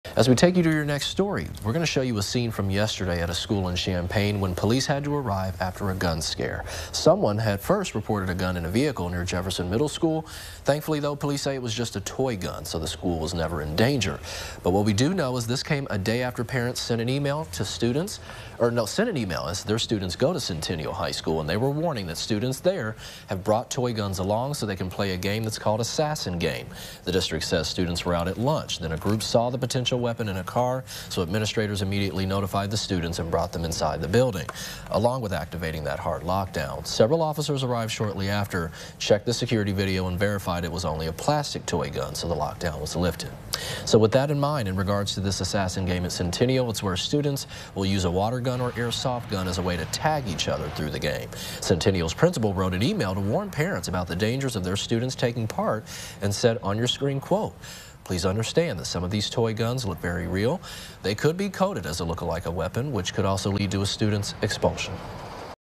Champaign middle school locked down after gun reported nearby
Jefferson Middle School in Champaign was placed on a hard lockdown Wednesday afternoon after students thought they saw a gun in a passing car